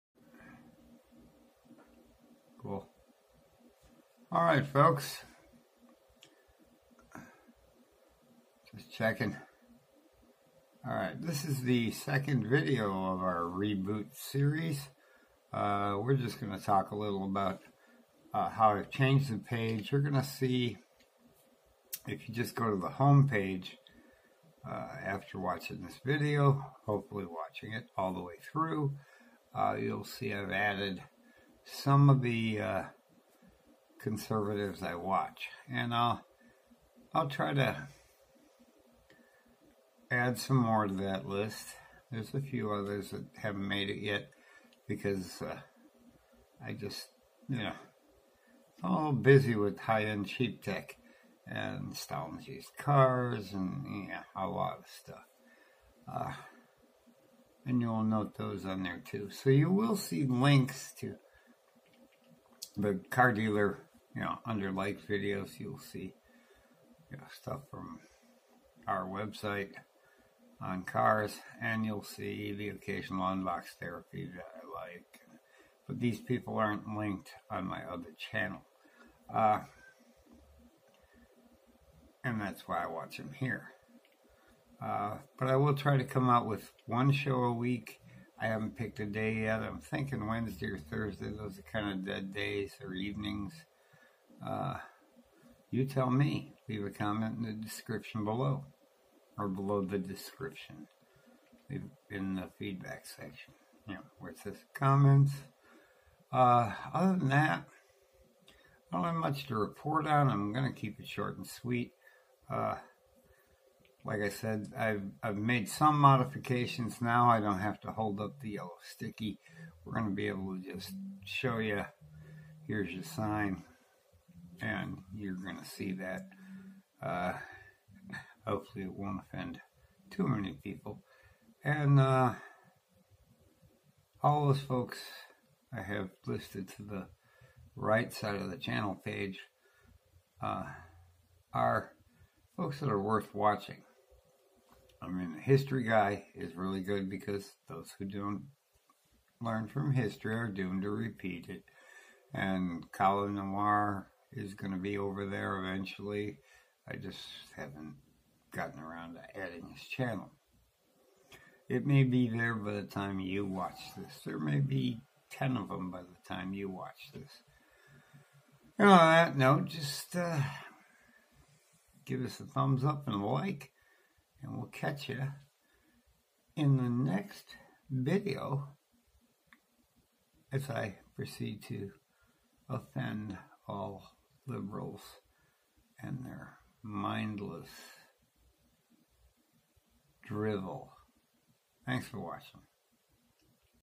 The TPC Reboot Episode 2
Tonight we have Episode 2 of my reboot of this channel. Not a whole lot going on but you will see some changes to the home page. I have changed the Who I watch section a bit and added my smiling face to the header.

Videos or links mentioned in this video:
none